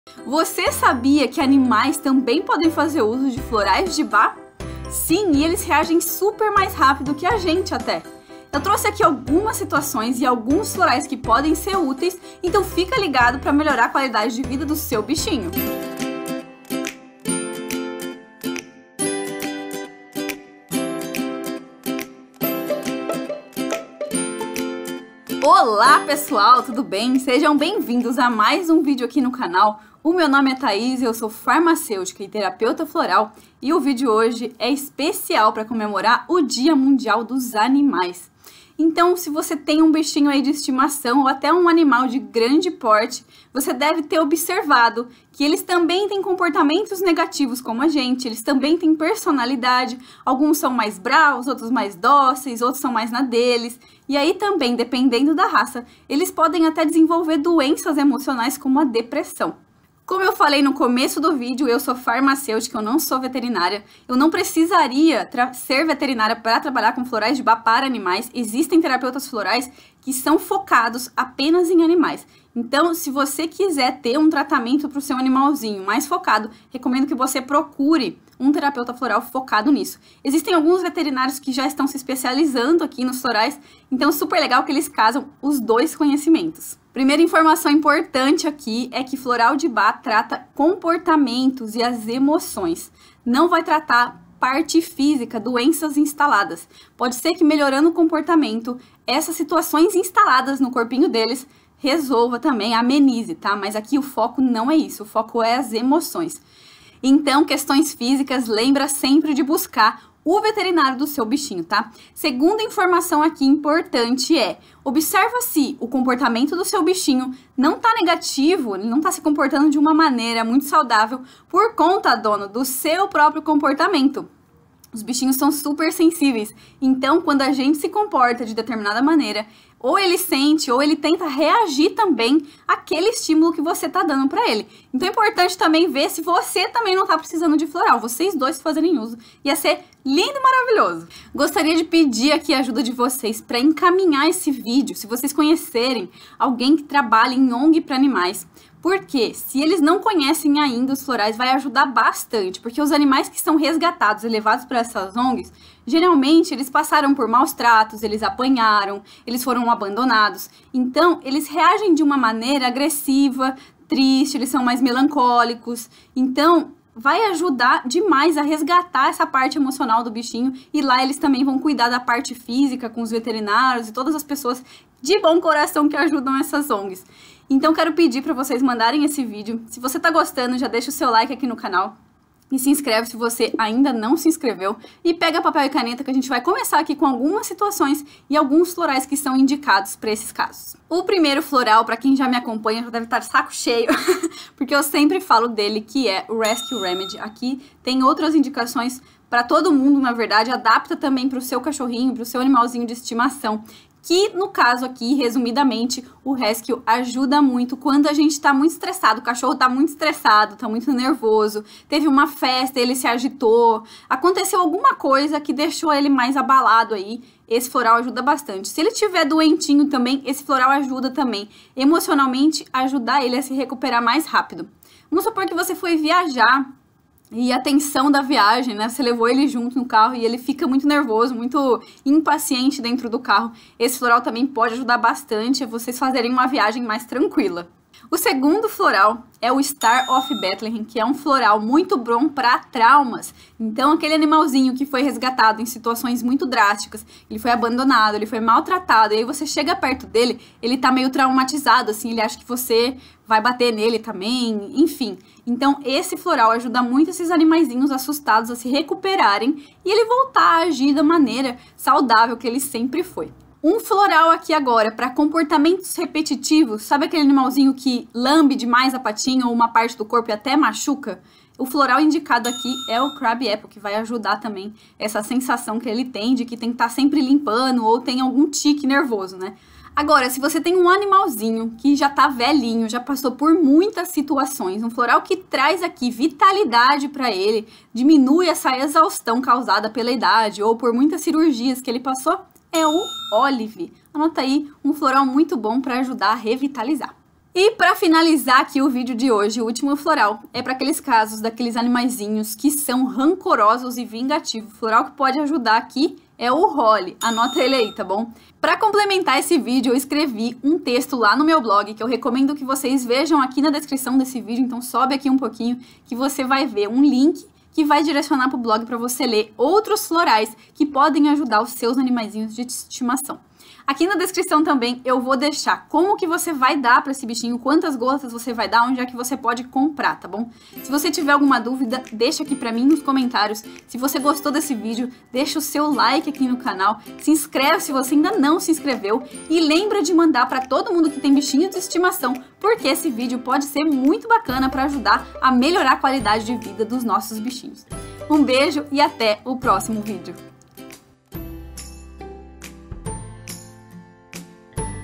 🌸 Florais de Bach para Animais 🌸 Thaís Scapim
Os animais podem fazer uso de florais de Bach e nesse vídeo eu cito alguns florais que podem ser usados em situações de estresse, de abandono, traumas, cansaço devido a muitas doenças ou idade e aqueles rancorosos e vingativos.

Como dar esses florais para animais:

- anotar os nomes dos florais que se enquadra com seu bichinho
- levar numa farmácia de manipulação homeopática e pedir pra manipular um frasco de 30 ml.
- Cachorros, gatos e outros animais de estimação: 4 gotas 4 x ao dia direto na boca, ou na comida ou biscoito para animais,  ou mesmo adicionar no bebedouro de água.
- Animais de grande porte como cavalo: 10 gotas no bebedouro ou 4 gotas no cubo de açúcar 4 x ao dia.

🌸 TERAPIA:
Para agendar seu horário e ter um atendimento exclusivo e personalizado com a Thá, acesse

📕Livros sobre florais

Lembre-se: Floral é tratamento complementar e não alternativo. Você pode continuar fazendo uso de outros medicamentos e associar os florais junto para equilibrar as emoções.

Thaís Scapim - Professora e Terapeuta floral dos sistemas de Bach

Se inscreve aqui no canal 😃